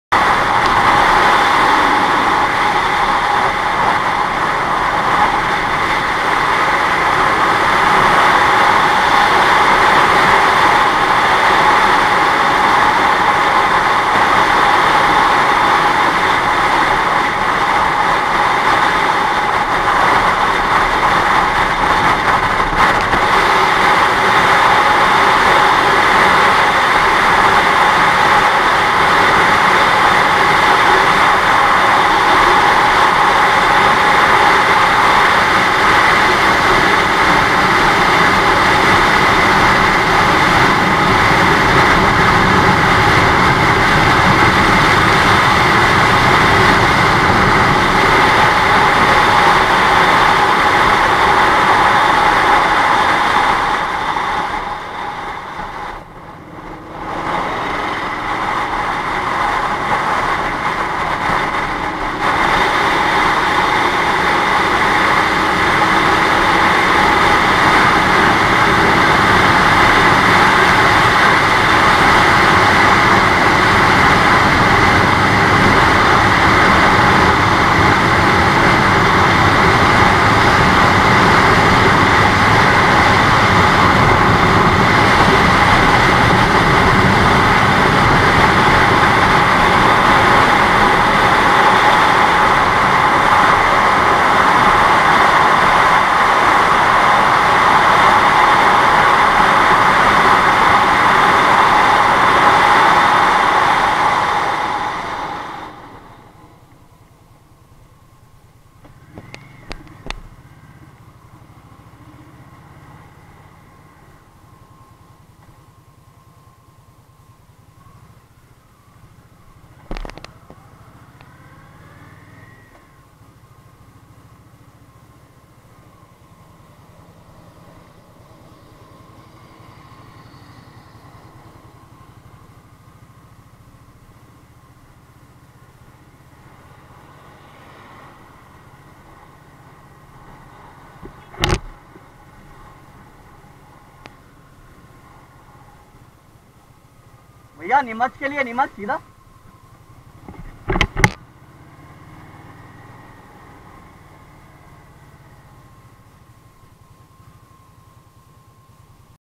2018 Indian Scout Sixty Top Speed | MAXED OUT!
Does 200 km/h and averages 25 km/l! The best fuel efficiency figure in my 1500 km ride I got was 26.79 km/l while the worst was 22.28 km/l. The average comes to 24.84 km/l! Astounding for high performance (for a cruiser) 999 cc engine!
Wet weight: 254 kg
Fuel Tank Capacity: 12.5 L
Engine Capacity: 999 cc
Torque: 88 Nm @ 5,000 rpm
Power: 77 hp (rpm not disclosed)
Rider Weight: 60 kg (Sixty on a Sixty, haha)